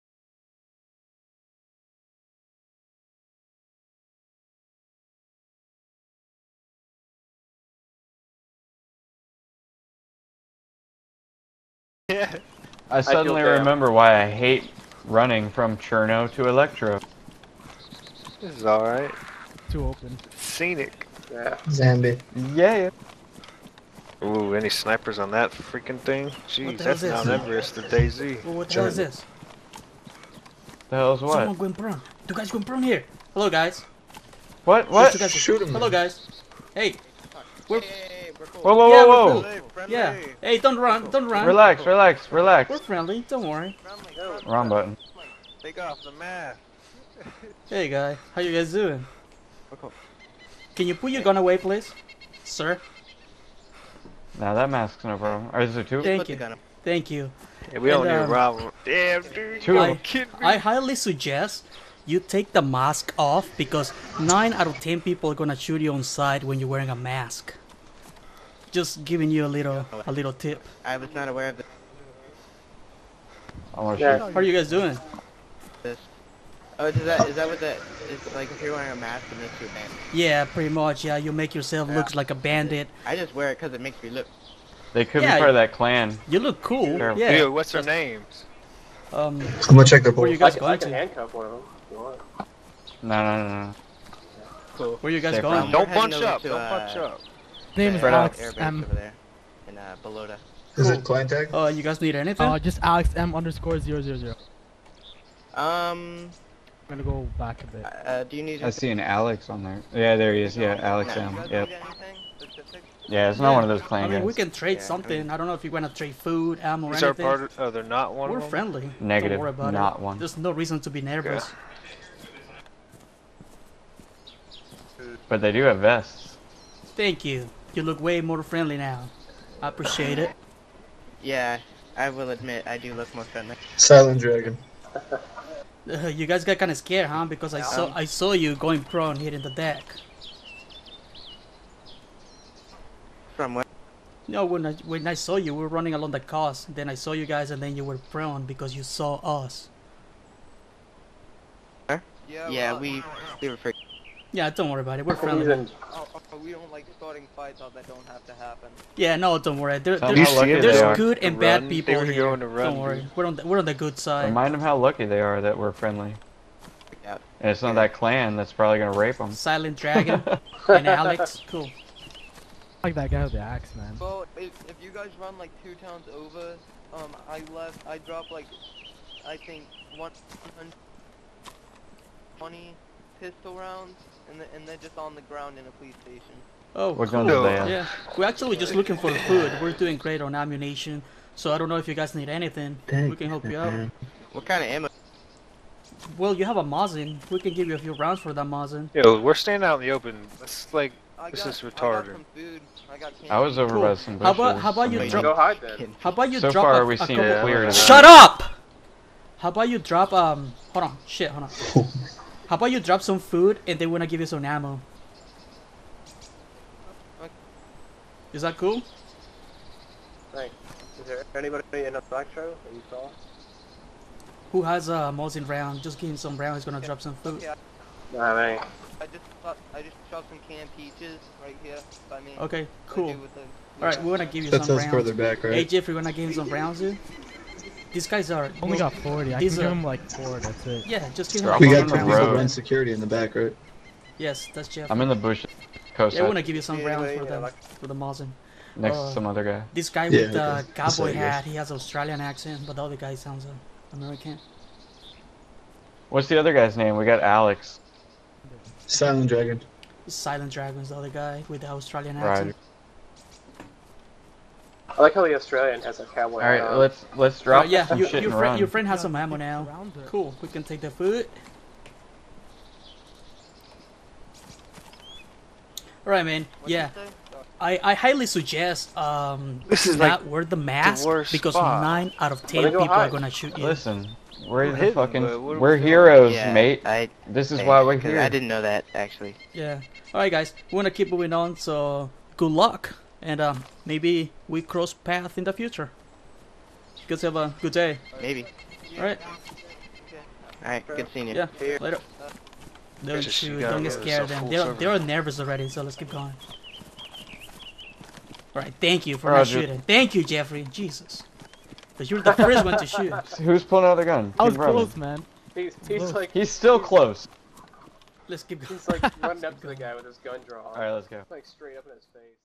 Road Encounter
We were moving from Chernogorsk to Elektro, then we spotted 2 individuals going prone and trying to hide from us.
Always, always, always!!! take a look around you at all times.